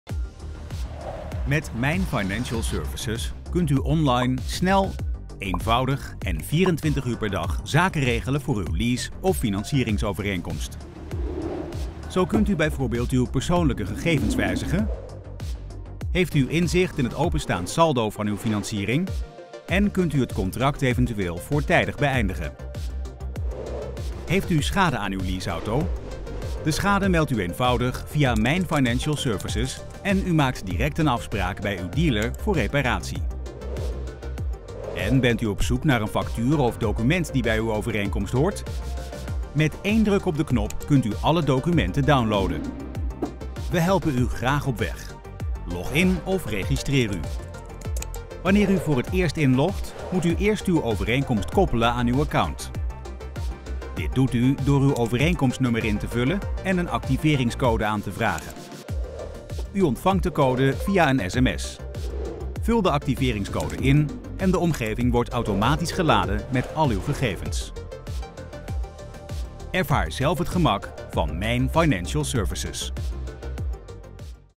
BMW Leasen en Financieren | Mijn Financial Services. Ervaar zelf het gemak. (BMW.nl)
Met Mijn Financial Services kunt u eenvoudig online uw lease-of financieringsovereenkomst van BMW Financial Services inzien. Bent u op zoek naar een factuur of document die bij uw overeenkomst hoort? Met enkele klikken kunt u alle documenten downloaden. Bovendien kunt u zelf uw persoonlijke gegevens beheren en heeft u altijd inzicht in het openstaand saldo van uw overeenkomst. En in het geval van een lease auto kunt u hier ook schade aan uw voertuig melden en het innameprotocol bekijken.

Kortom: alle informatie over uw overeenkomst kunt u 24/7 raadplegen en wijzigen in Mijn Financial Services. Wel zo gemakkelijk! Ontdek het zelf.